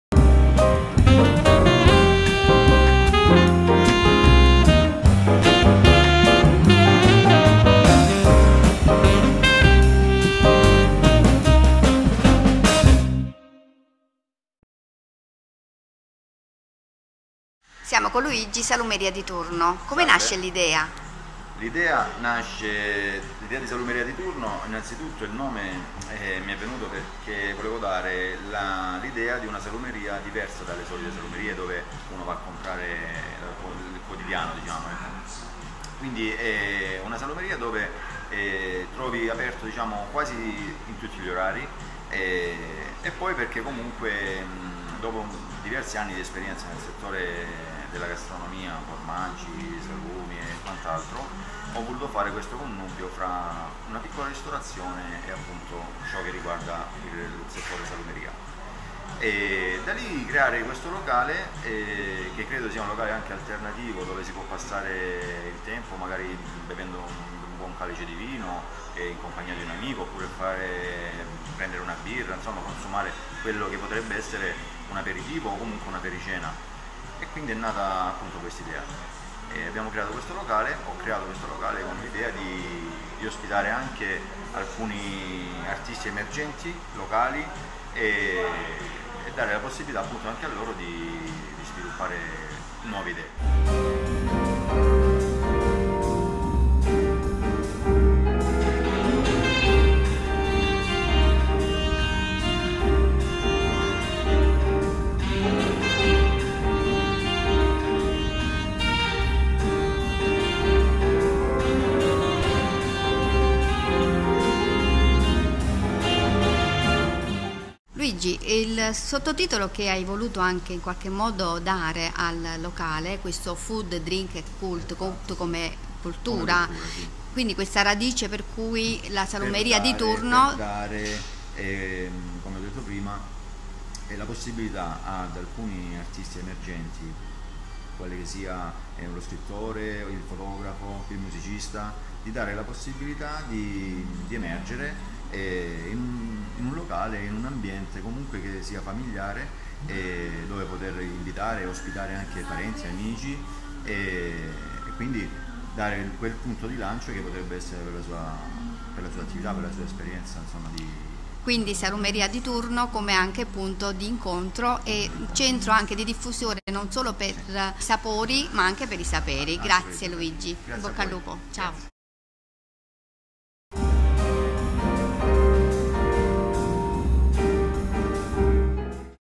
Salumeria di Turno
Salumeria di Turno Food drink cult  la nuova idea di ristorazione e intrattenimento culturale ideata da Luigi Solida. Nella breve intervista conosciamo questo giovane imprenditore di Galatina.